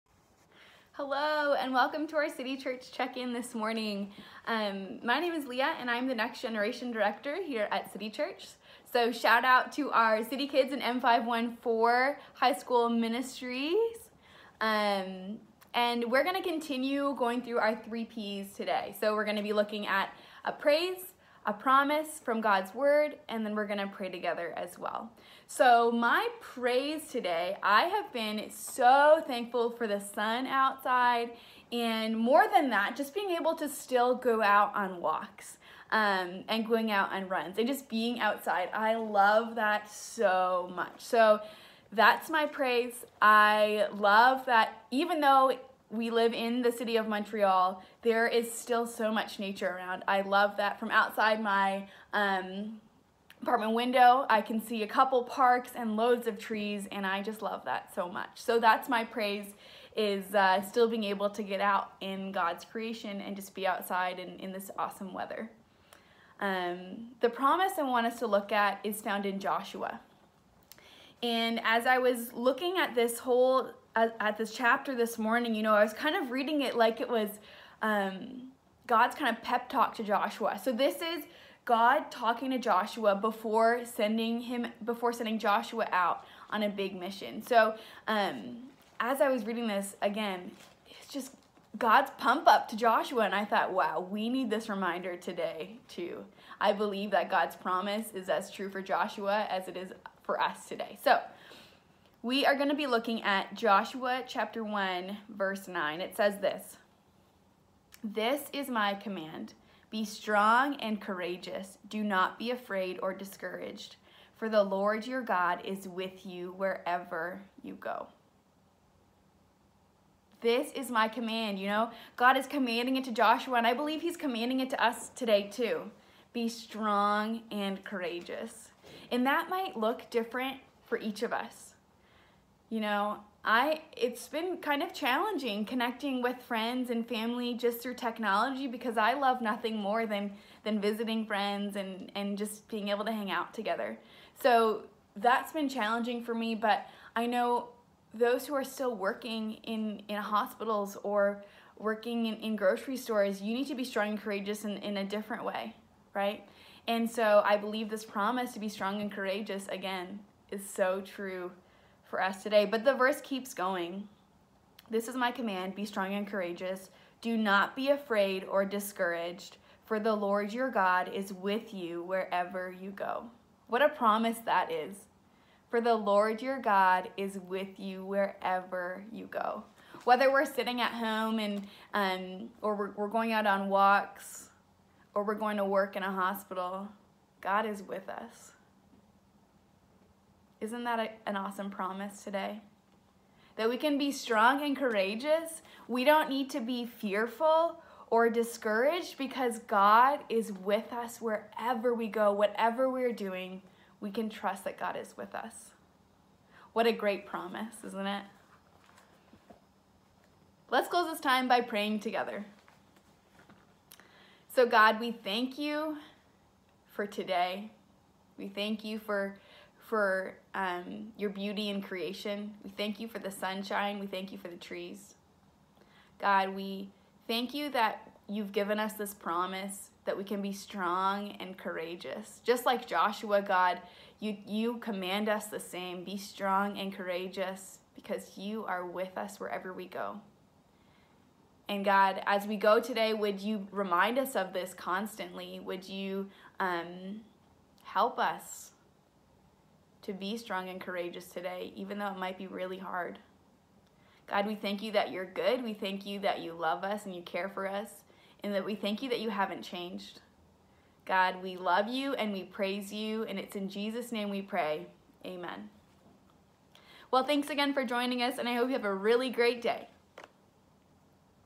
City Church Check-In: Thursday, April 2
Welcome to Day #4 of our City Church Check-In.

Each weekday, a different CITYZEN of City Church will be sharing 3 Ps: Praise, Promise and Prayer.

Today's promise is from Joshua 1:9 and is so important for us to remember in these days of fear and uncertainty.

Feel free to write in your own favourite promise to hold onto.